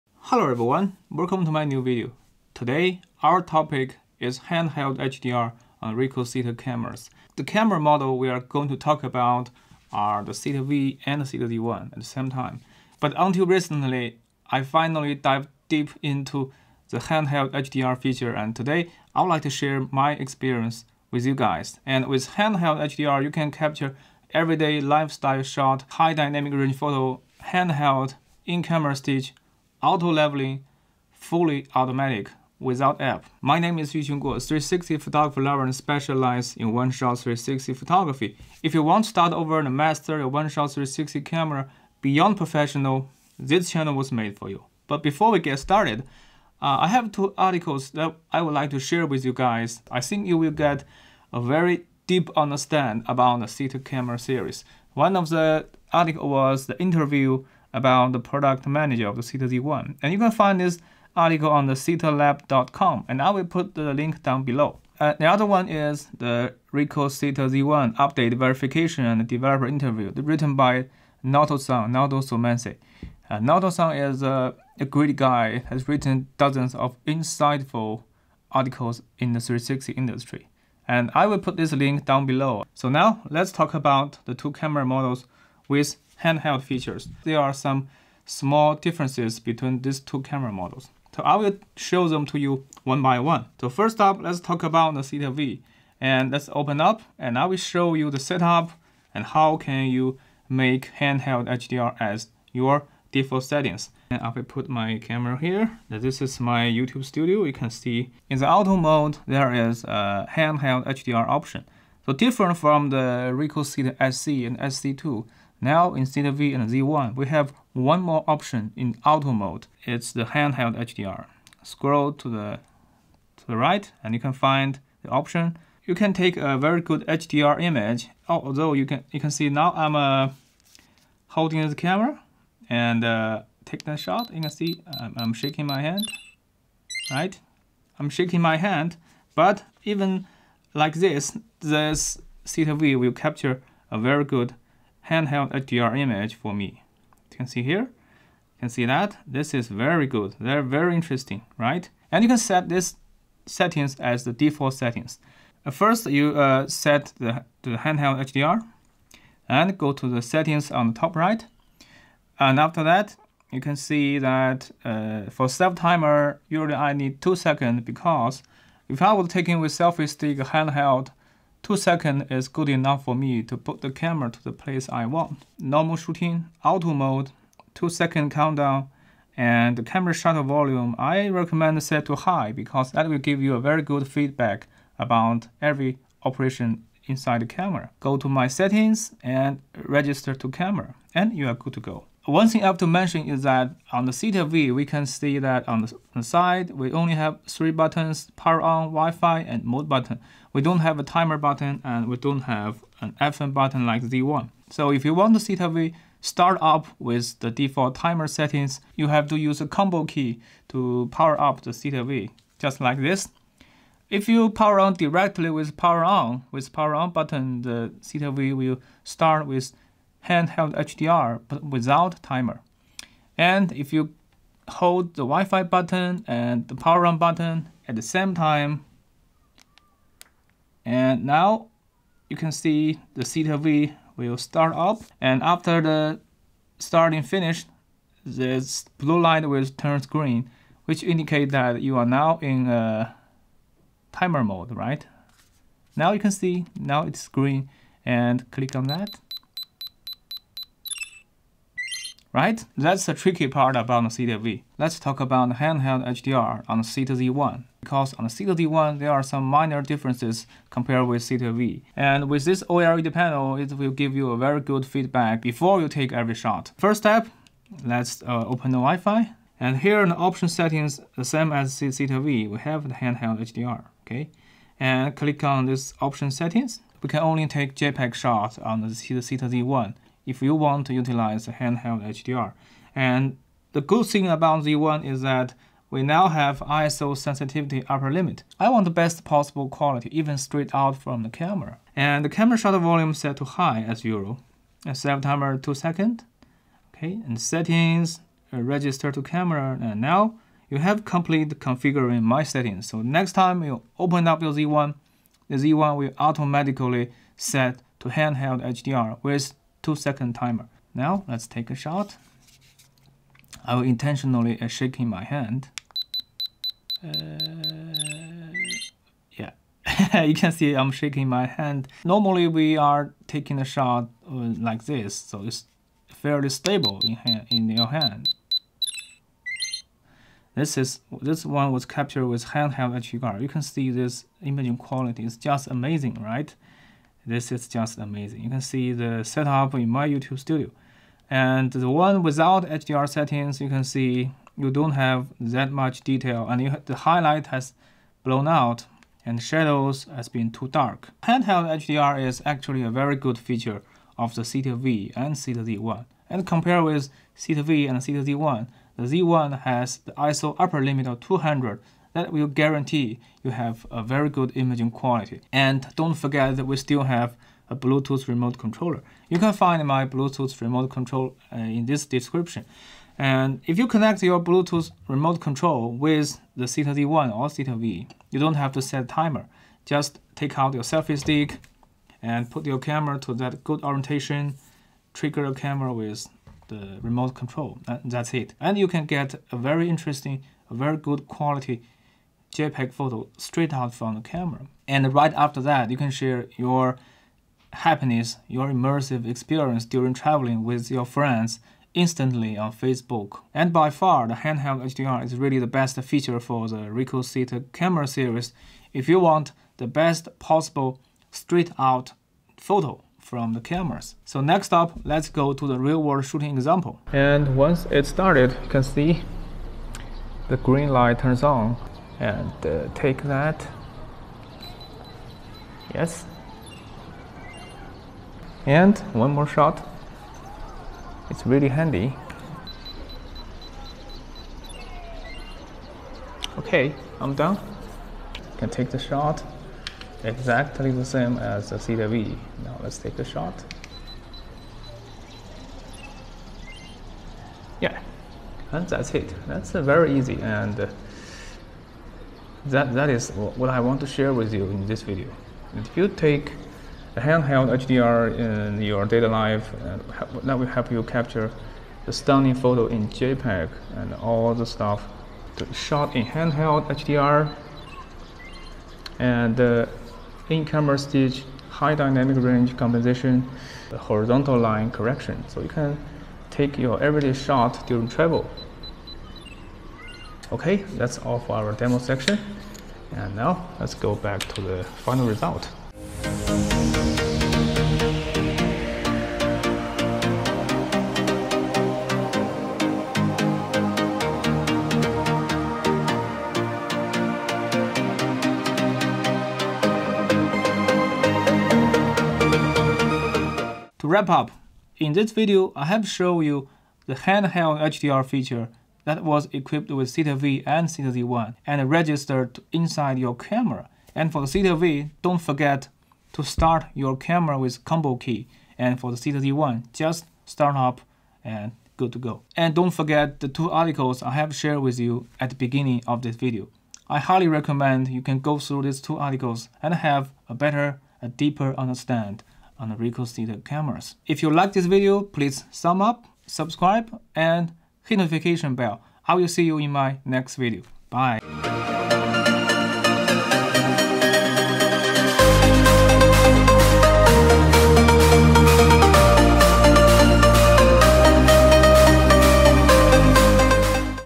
Ricoh THETA Handheld HDR Explained!!!
I will show you how to make the most of Handheld HDR feature from THETA V and THETA Z1. And I will also tell you the difference on this interesting feature between these two cameras.
Time code
00:48 Two Recommended Articles from THETA Lab and VideoSalon
01:23 Handheld HDR on THETA V
03:15 Combo Key to start with Timer on THETA V
04:15 Handheld HDR on THETA Z1
06:55 Real World Shooting Example
08:05 Final Result and Wrap Up

Recommended Articles

Interview with the THETA Z1 Project Manager about Planning and Development

Ricoh THETA Z1 update verification & developer interview
by Naoto Somese